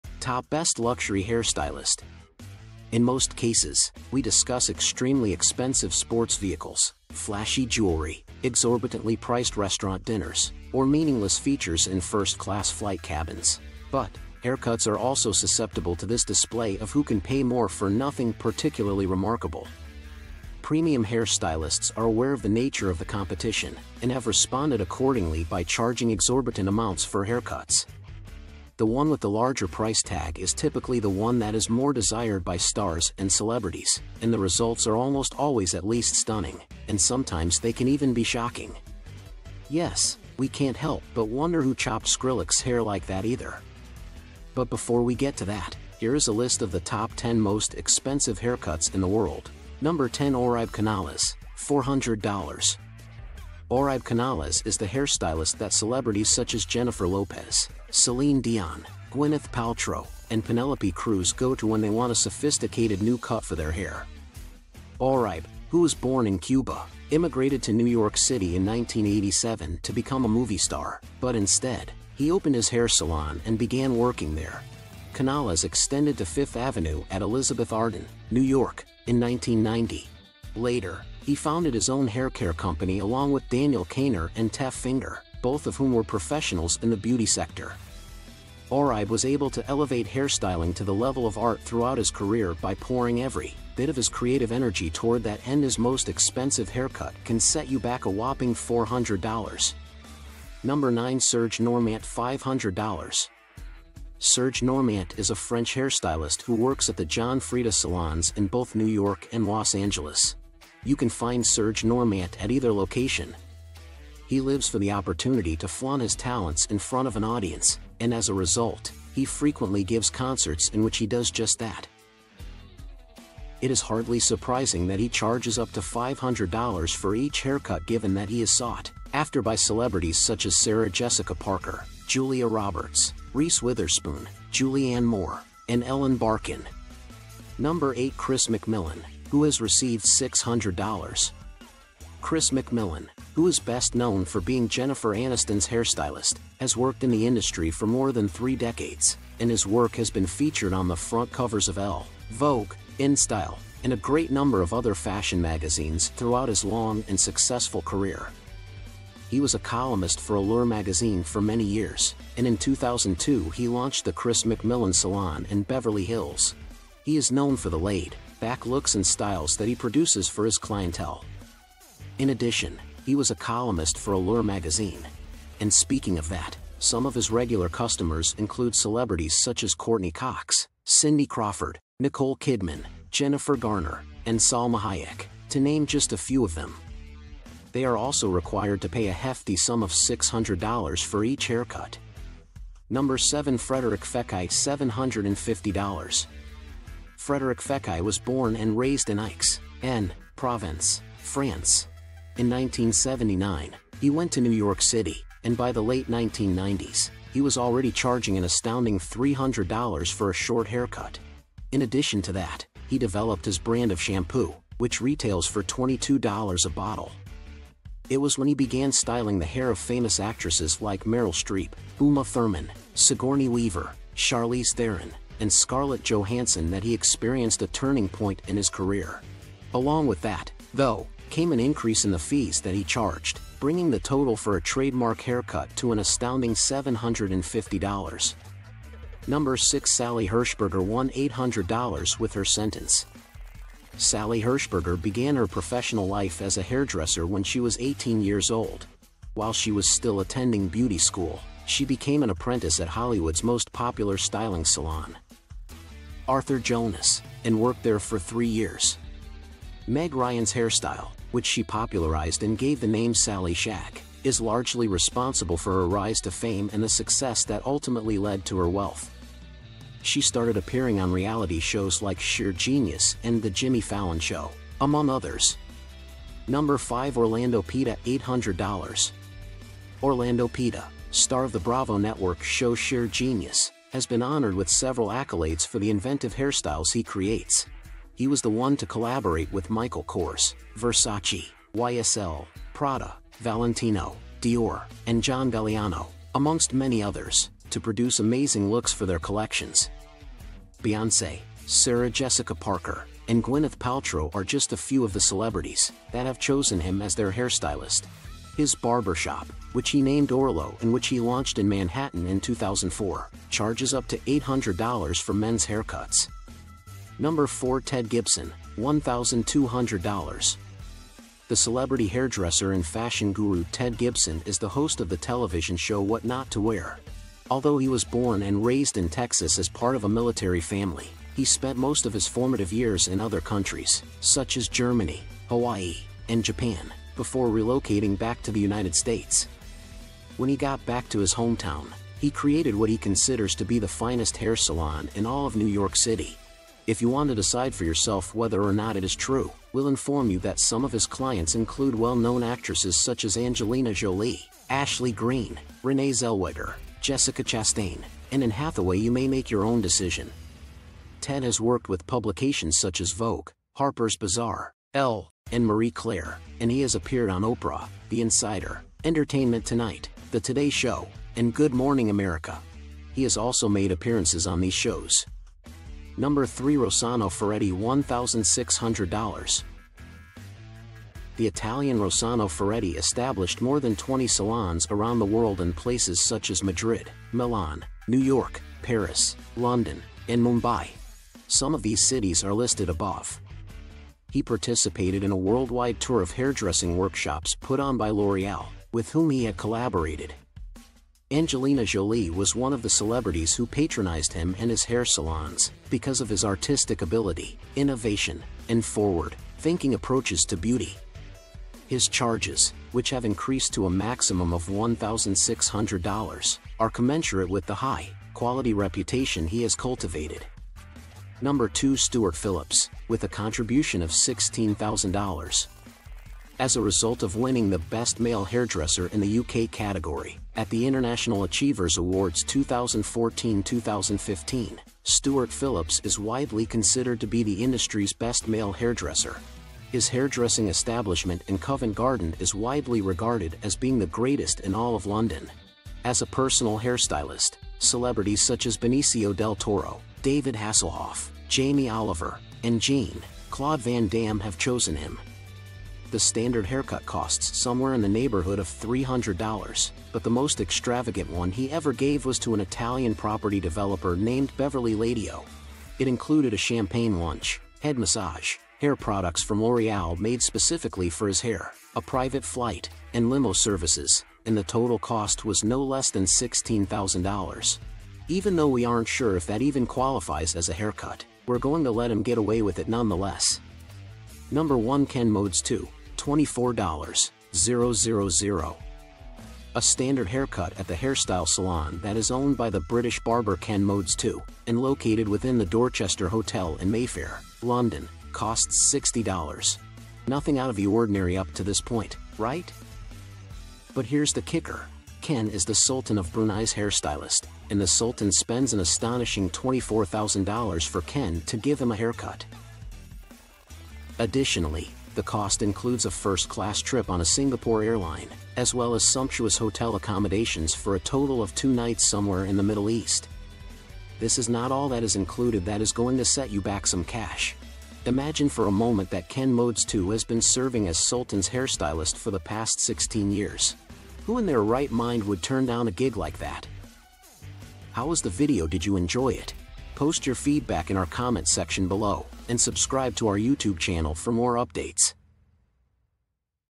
Top best luxury hair stylist
Welcome to our latest video on the Top Best Luxury Hair Stylists. In this video, we will take you on a journey into the world of luxury hair styling and introduce you to some of the most talented and sought-after hair stylists in the industry.

These top luxury hair stylists have worked with some of the biggest names in the entertainment industry, from runway shows to celebrity clients, and have become renowned for their unique and innovative styles. We will showcase their skills and expertise and share some of their most iconic hairstyles that have become the inspiration for many.

Throughout the video, we will also highlight the latest trends and techniques being used in the world of luxury hair styling, and provide tips and advice on how you can achieve the perfect hairstyle.

If you're a hair enthusiast, a stylist or simply someone who loves luxury beauty, then this video is the perfect place to discover some of the best hair stylists in the industry and explore the world of luxury hair styling.